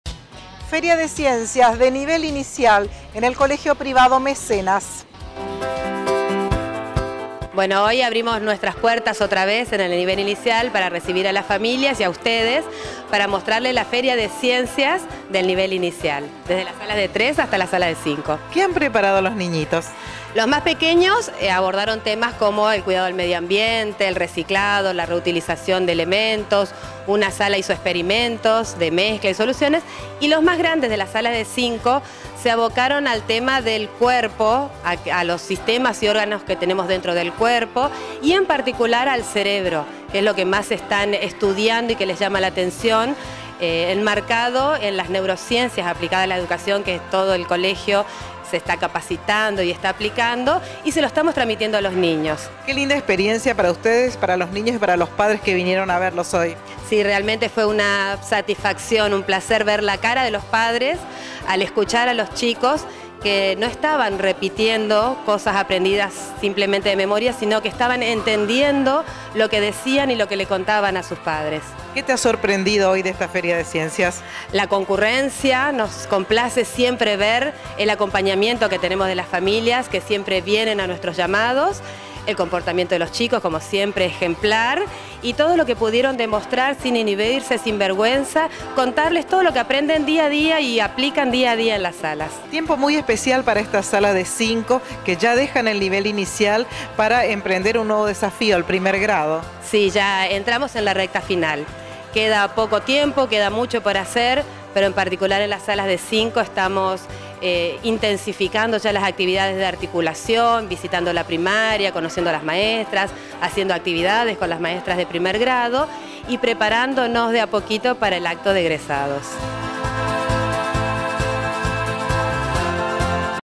MECENAS Colegio Privado
Feria de Ciencias de los pequeños del Nivel Inicial . . .